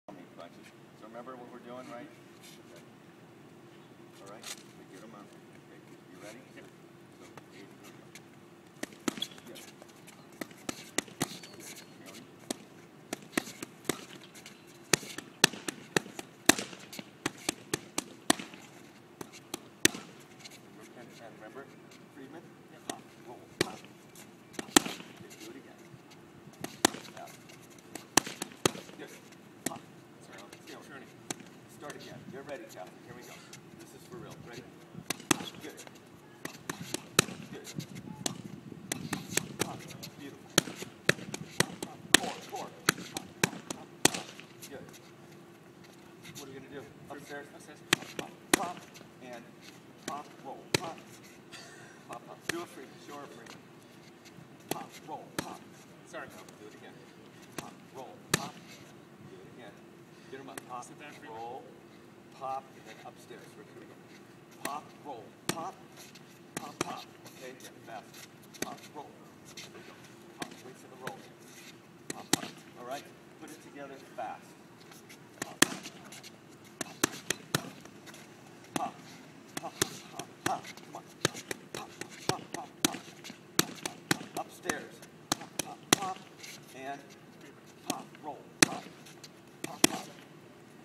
Deacon and Phil boxing
Montecito Heights Health Club boxing phenom Phil Krohn teaches Deacon Carpenter how to hit with mitts.